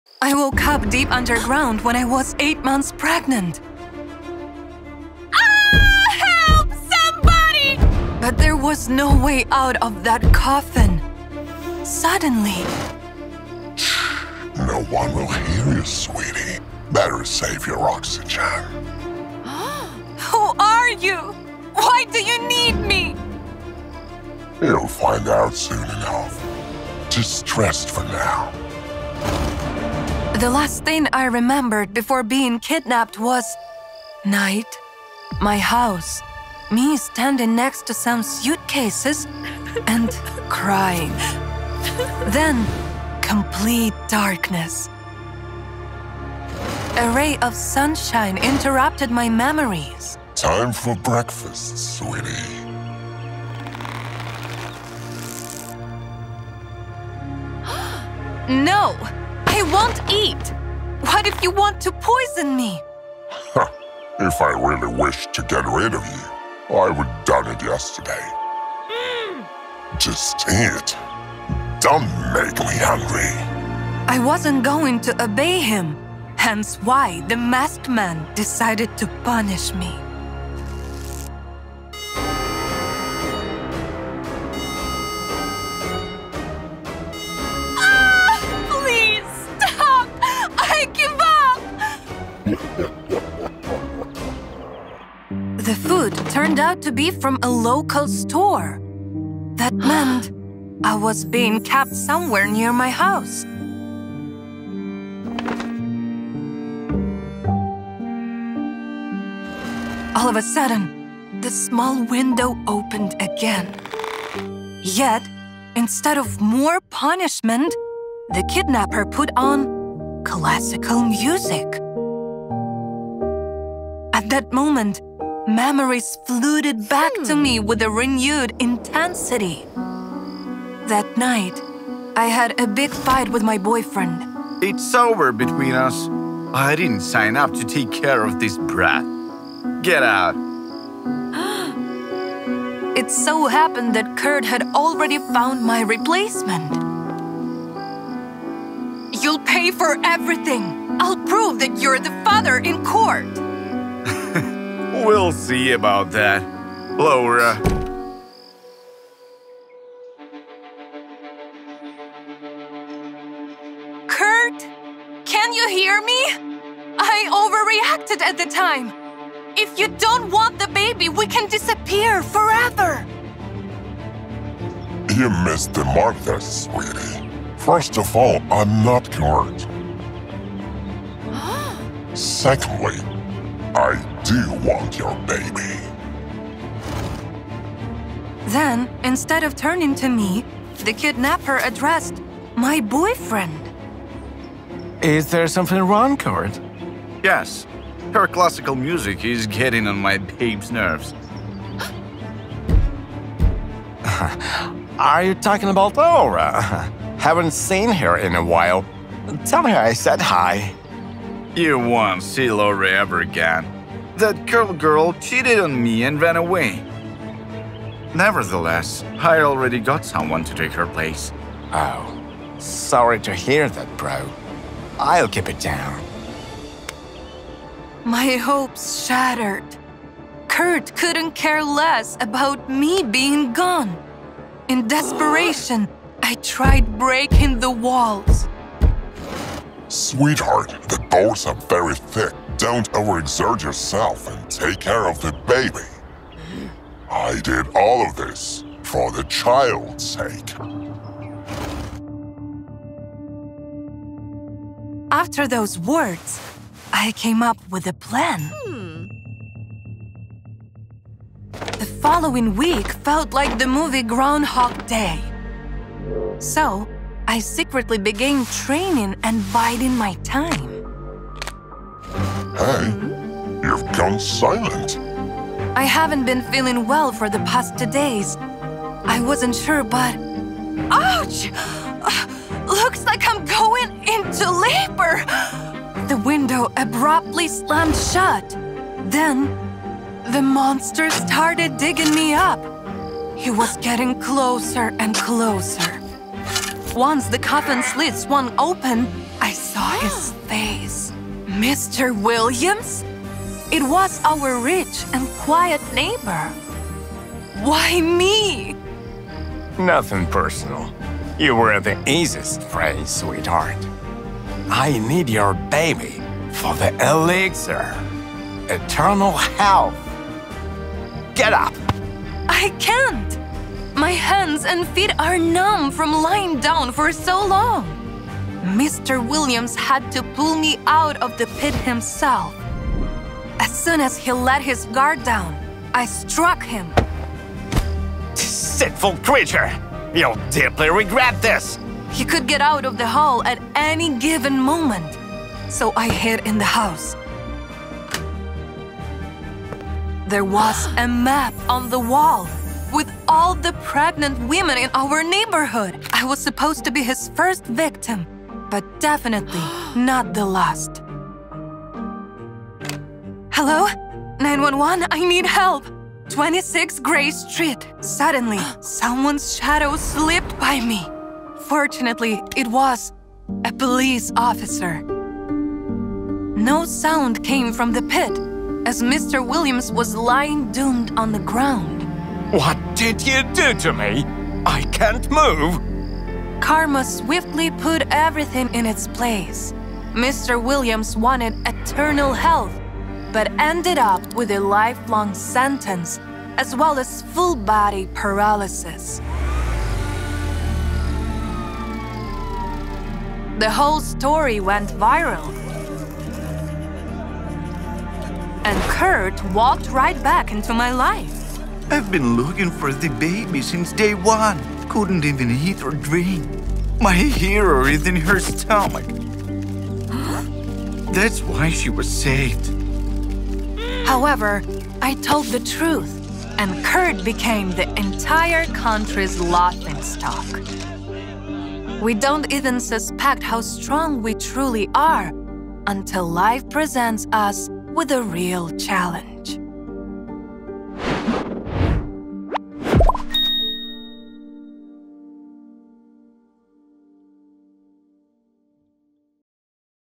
I Woke Up Underground When I Was Eight Months Pregnant
Are all bad things that happen to us a challenge to our character?

Laura, at eight months pregnant, woke up one night and realized that she was in a box buried underground. She began to scream and bang on the walls. Suddenly, a stranger in a mask showed himself through a thin tunnel that connected Laura’s “coffin” to the outside. He ordered her to keep quiet, as no one would hear her anyway.

And when Laura asked why the man did this to her, he said that he needed her baby.

➜ Feel free to take a look at our other projects to treat yourself to soul-stirring stories, all-about-love short films, and special edition episodes: